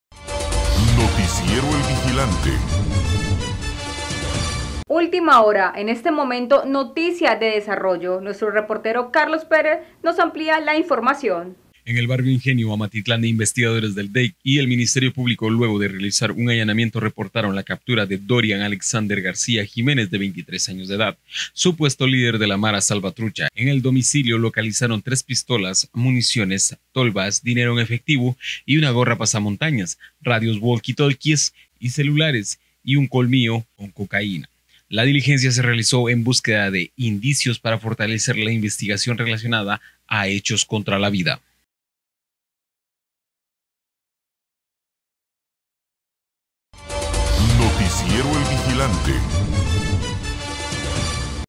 Supuesto líder de mara salvatrucha capturado en allanamiento
En el barrio El Ingenio, Amatitlán, investigadores de la DEIC y el MP luego de realizar un allanamiento reportan la captura de Dorian Alexander García Jiménez, de 23 años, supuesto líder de la Mara Salvatrucha, en el domicilio localizaron tres pistolas, municiones, tolvas, dinero en efectivo, un gorro pasamontañas, radios Walkie Talkies, celulares y un "colmillo" con cocaína.

La diligencia se realizó en busca de indicios para fortalecer la investigación relacionada a hechos contra la vida.